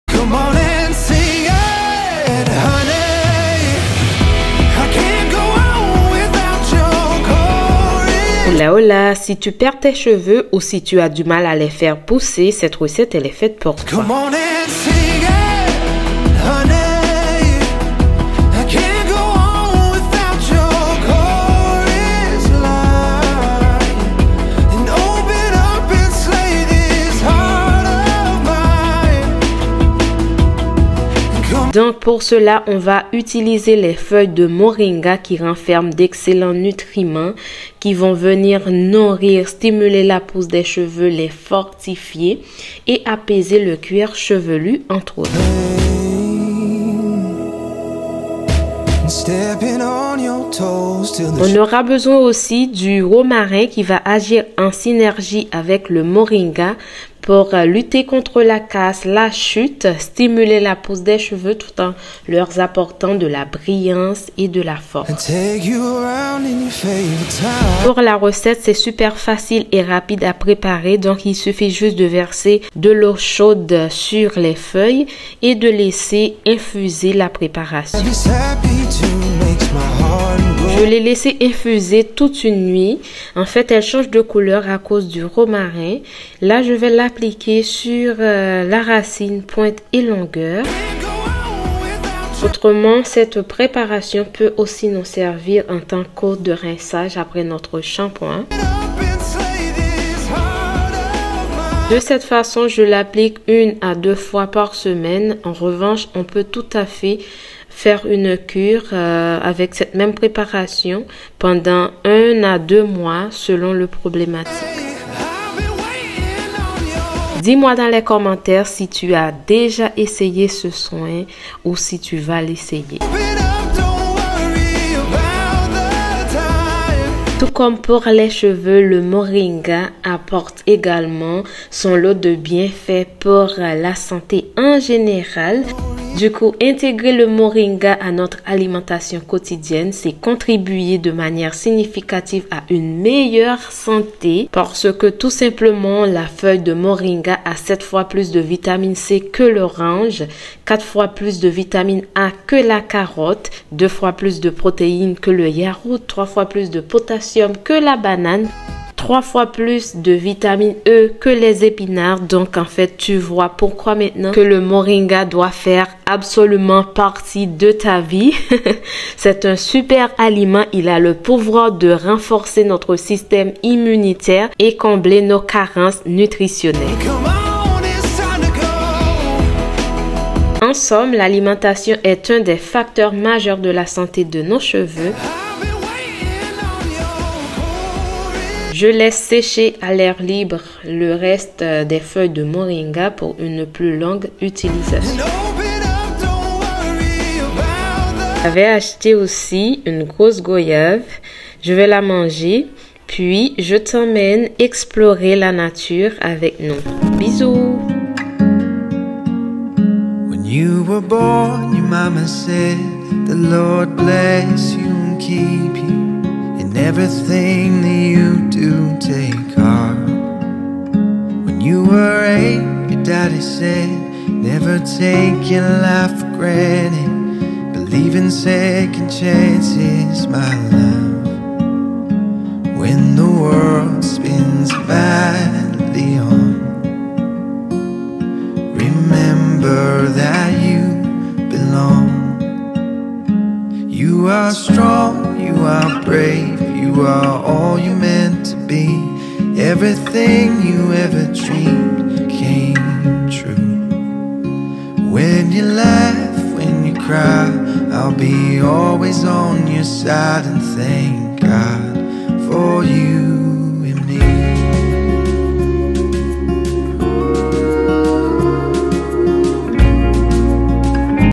SPRAY CHEVEUX |MORINGA|ROMARIN
Hola! Hola !

Dans cette vidéo je te montre comment j’utilise le moringa pour fortifier et faire pousser les cheveux plus rapide.